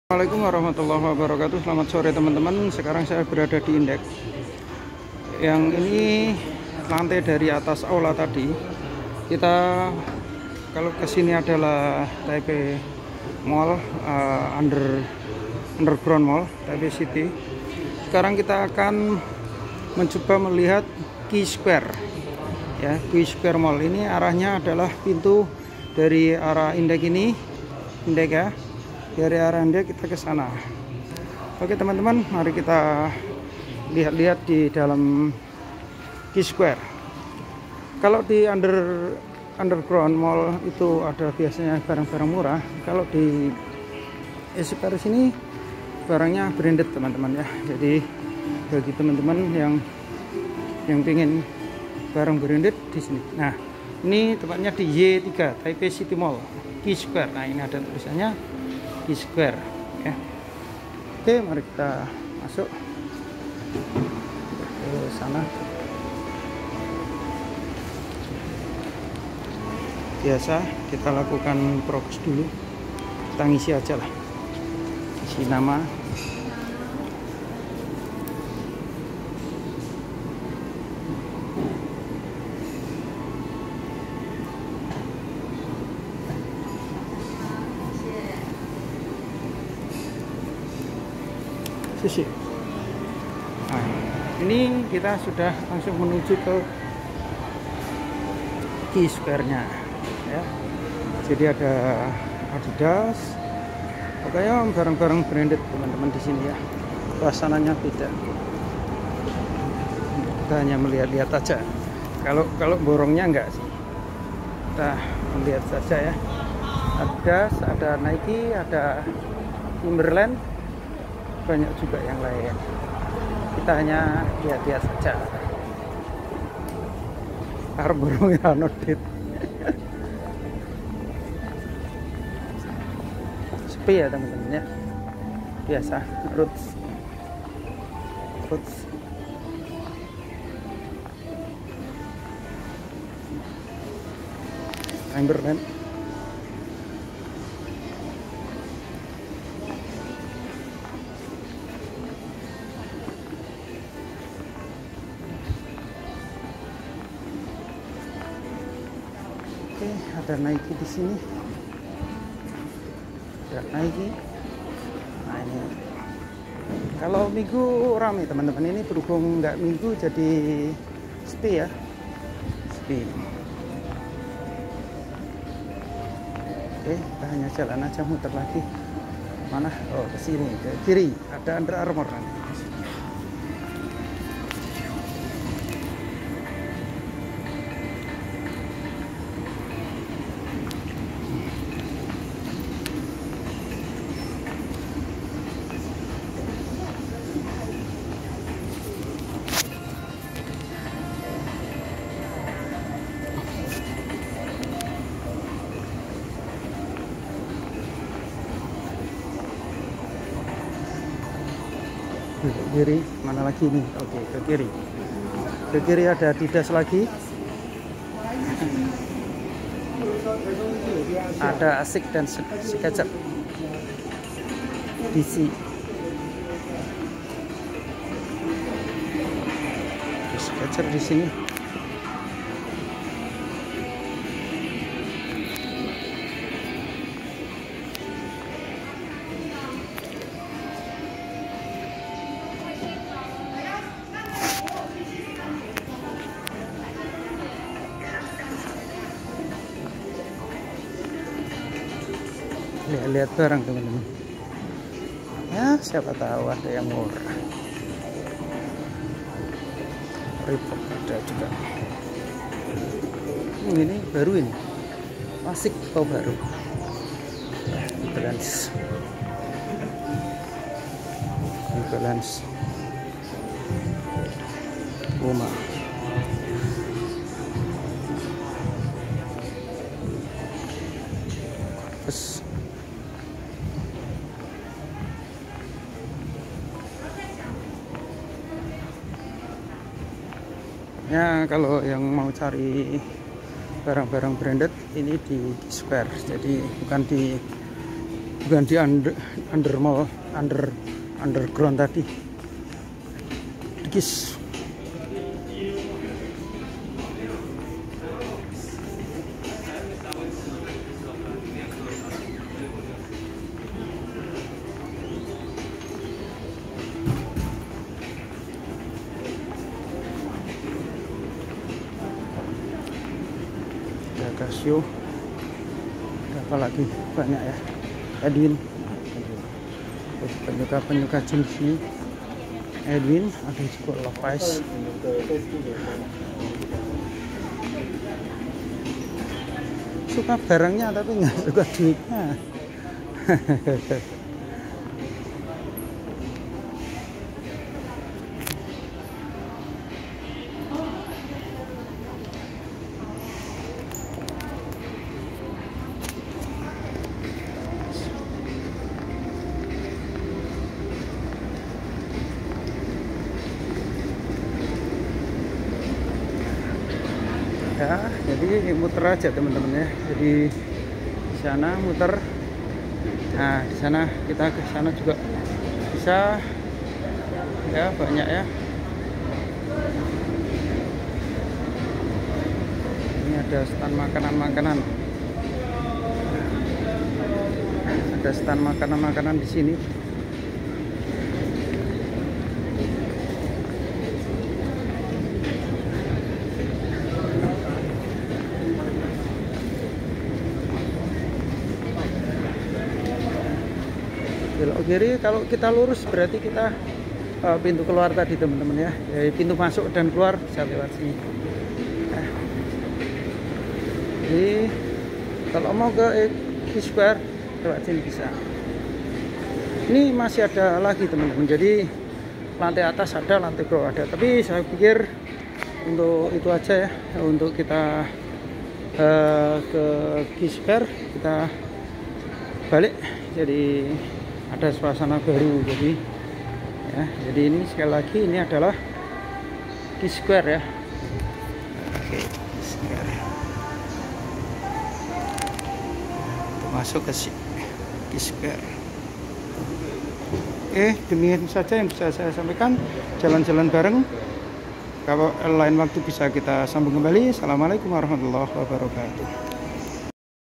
Q-SQUARE MALL TAIPEI || TAIPEI CITY MALL
Q-square  merupakan salah satu mall besar yang berada dibawah tanah, tepatnya di bawah TAIPEI CITY BUS STATION ( terminal nis antar kota taipei), letaknya bersebelahan dengan Taipei City Mall / Taipei under ground mall. Didalamnya dijual bermacam barang branded dunia. Merupakan tempat yang cocok bagi yang hoby shopping atau hanya sekedar berjalan dan melihat saja.
Route kesini bisa ikuti link dibawah